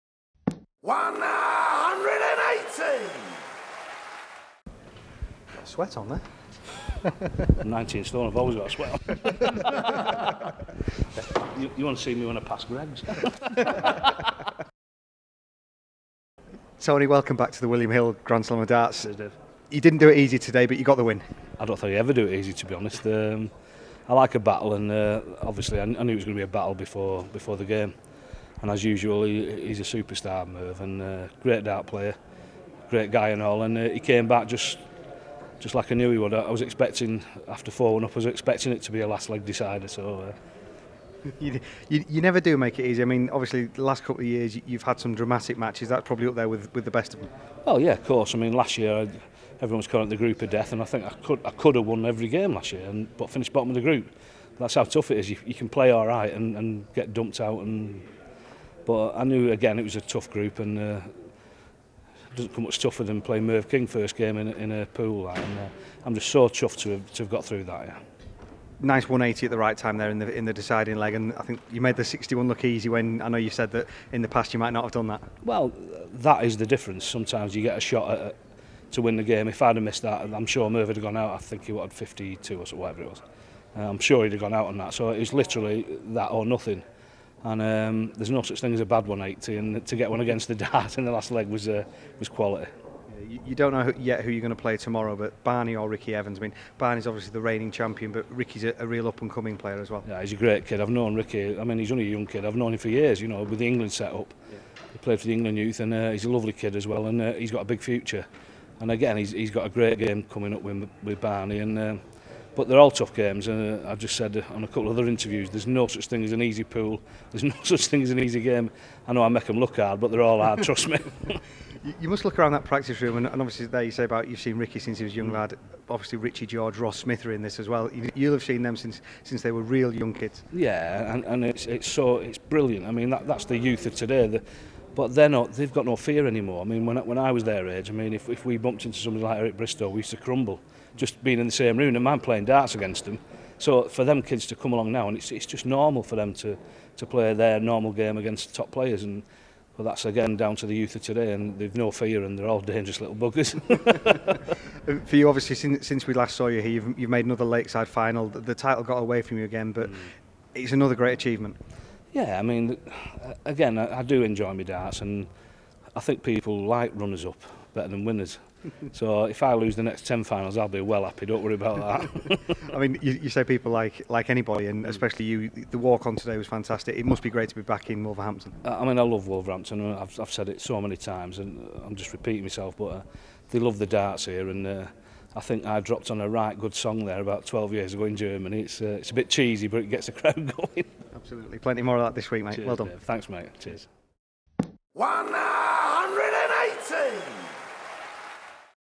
Tony O'Shea wins his first group match at the William Hill Grand Slam of Darts
Official PDC brings you interviews, highlights, exclusive footage and much more from the World of PDC Darts !